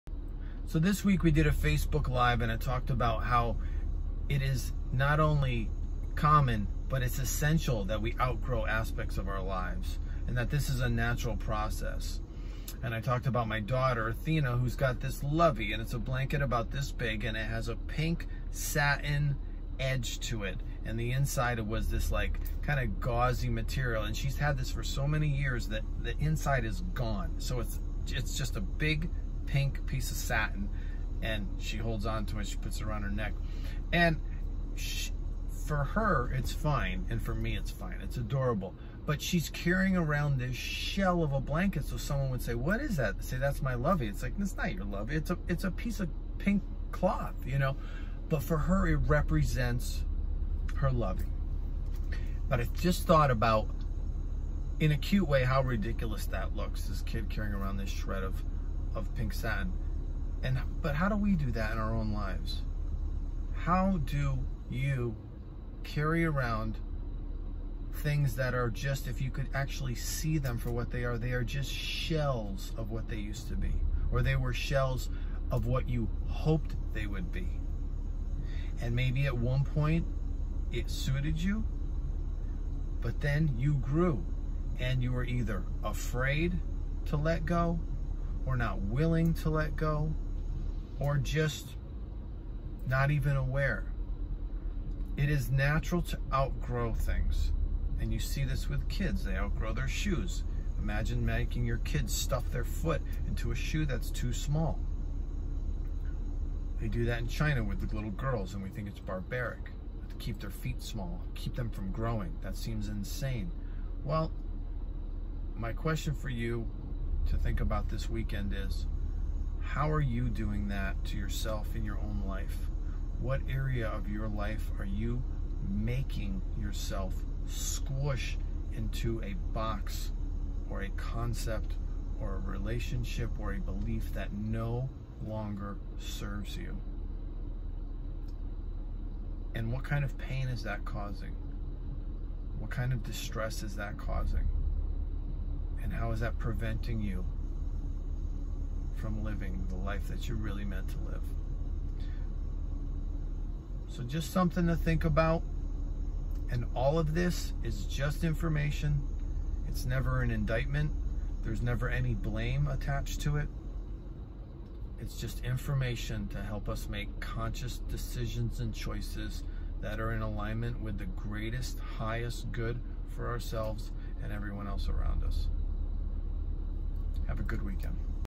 HAVE YOU OUTGROWN YOUR LIFE? Matt Starr Coaching
We outgrow shoes. We outgrow clothes. But we also outgrow beliefs, relationships and situations. This is not only natural. It is essential.